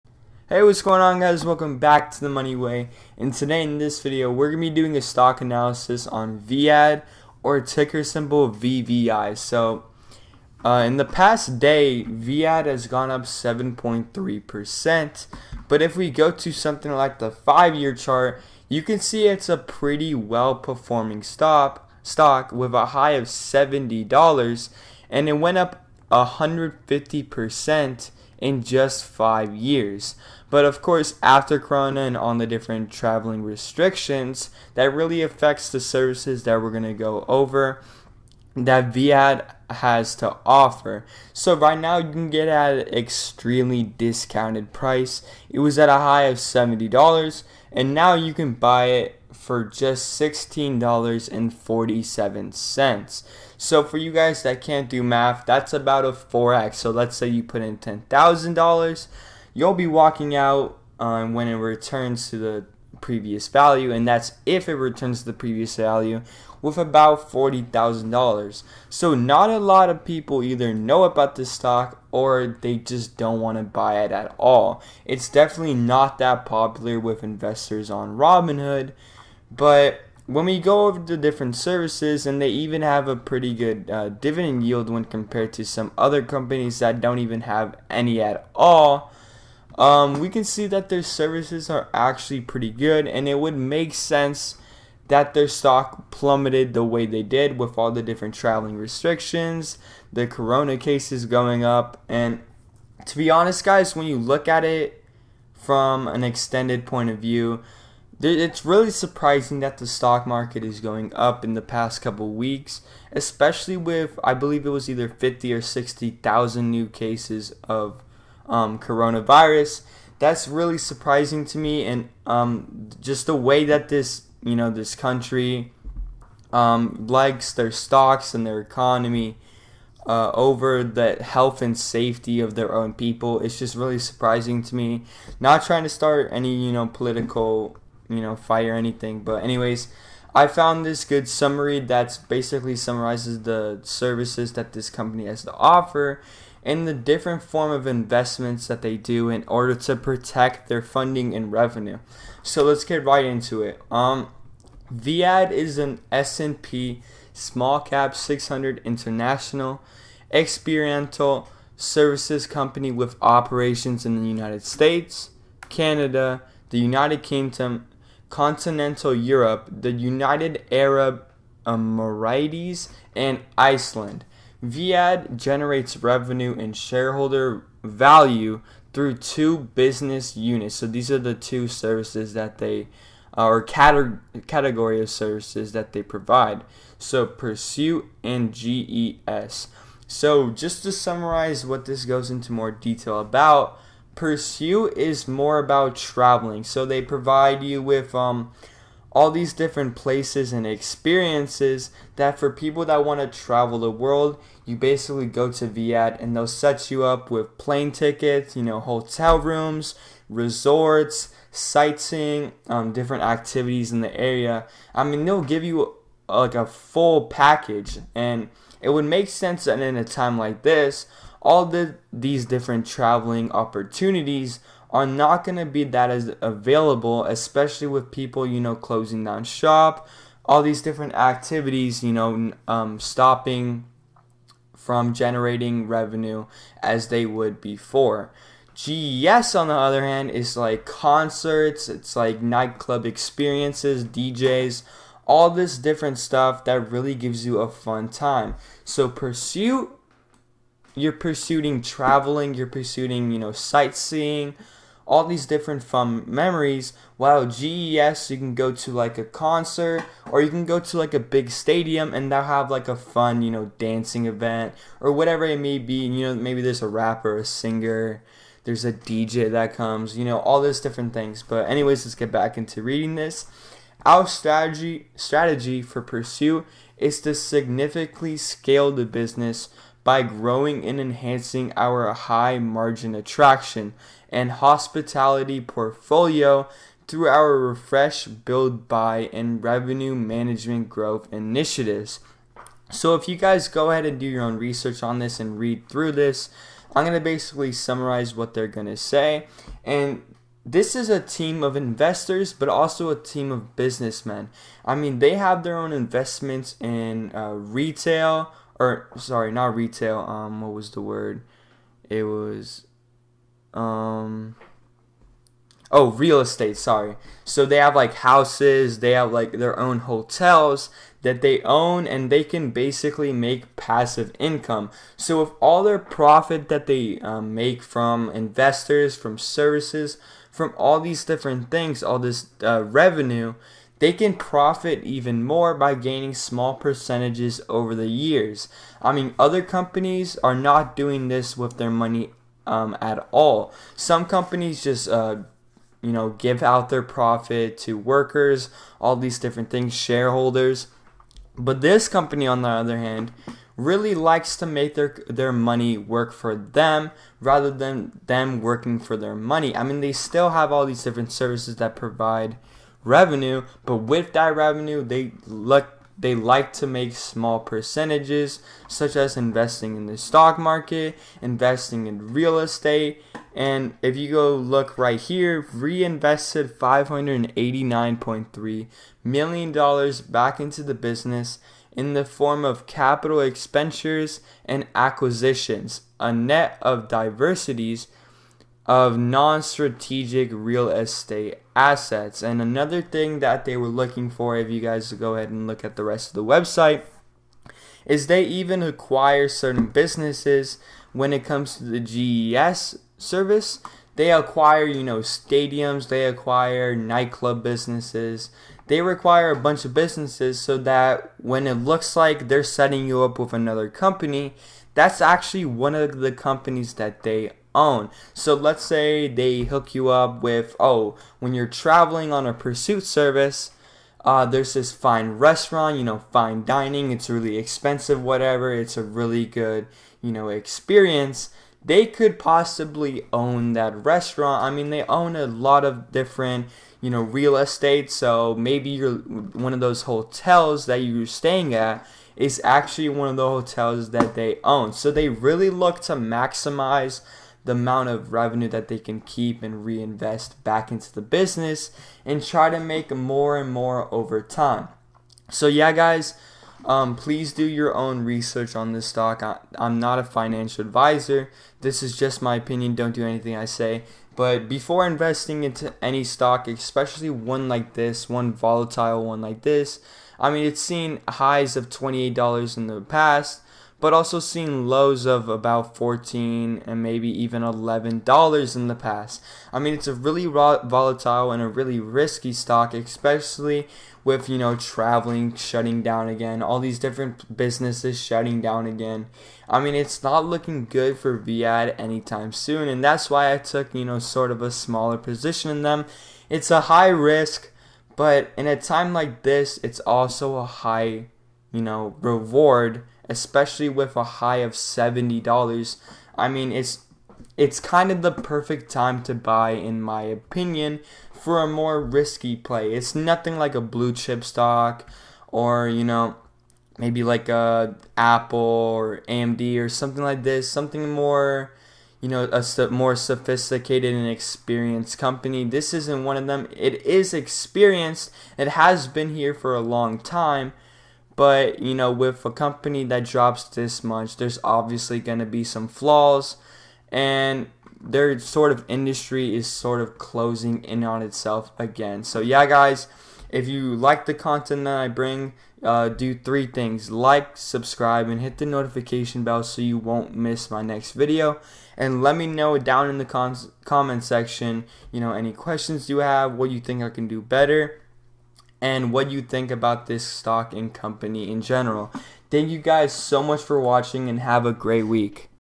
Viad Corp HIGH REWARD Stock Analysis | VVI Travel/Entertainment Stock Analysis
Hey what's up guys, this video is a stock analysis on Viad or VVI. This stock is overall one of the more risky stocks in my portfolio. They have a wide variety of services and also a wide variety of investments and properties that help them secure more revenue over the long term. This stock has fell over 75% in the past year, due to travel restrictions and the corona virus pandemic that has really messed things up for Viad and other companies in general.

Get one FREE STOCK on signup with robinhood using this